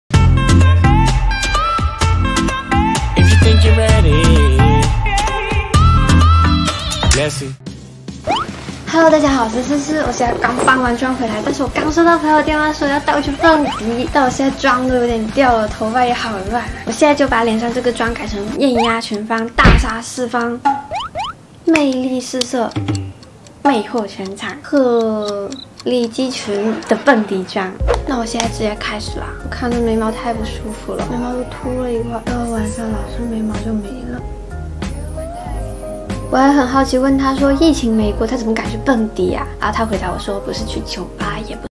【陈斯文cissie】朋友约我蹦迪居然。。。！？| 日常通勤妝秒变蹦迪妆slay全场！
下班接到朋友电话说要带我去蹦迪？！
把日常妆改造成蹦迪妆也能slay全场！！
日常大地色叠涂超美苍蝇绿～非日常的超浓假睫毛！
朋友居然跟我说不是去夜店也不是去趴体？到底是什么惊喜？！？！
来跟斯斯一起跳舞吧～